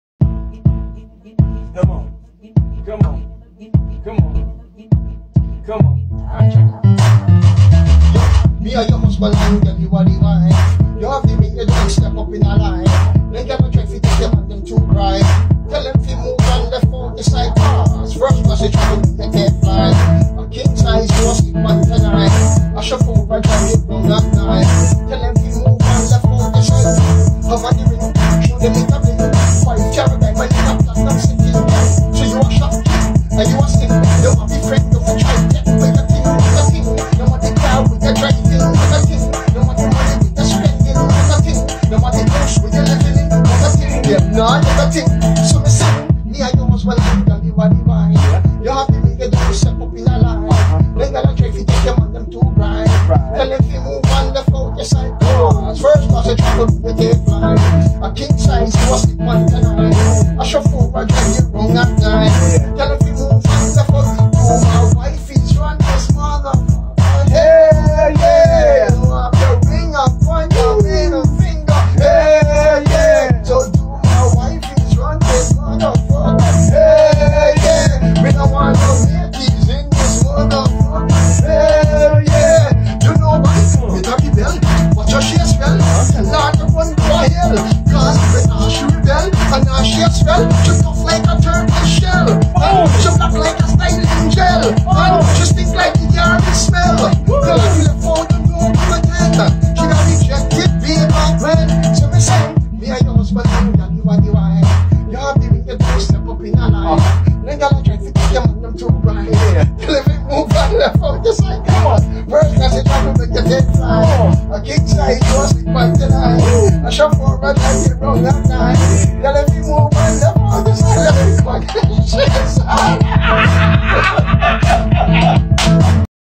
E.G. in studio working on new music
Dancehall artist E.G. in studio working on new music

Maria Jackson Entertainment
Publicity & Media